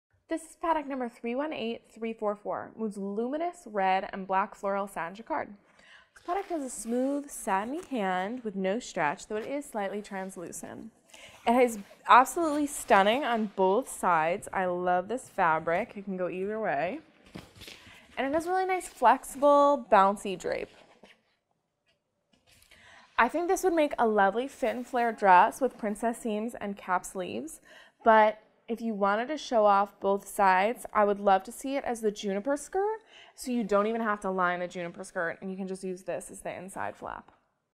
Mood Fabrics 318344 Luminous Red and Black Floral Satin Jacquard
A bit dramatic and oh, so romantic, this Luminous Red and Black Floral Satin Jacquard is sure to make jaws drop. A Victorian-inspired floral covers a luminous black satin background in a deep, luxurious tone. Incredibly smooth and thin, create sleek evening wear for any event. Its drape is flexible and falls into a fuller volume to create stunning gowns and cocktail dresses. Make backless and strapless princess silhouettes with ease! Lightweight, it is slightly translucent and may require a lining.
Color Family BLACK, RED
Content 100% Polyester
Care Instructions HOW TO CARE FOR POLYESTER FABRICS:
Machine wash warm or cold, tumble-dry low. If ironing is needed, iron on a low temperature.

Pantone Color BLACK CP, 202 CP
Color Black/Red
Width 58"
Returnable No
Categories
All / Pantone Collection / Intricacy
All / Fashion Fabrics / Polyester / Jacquard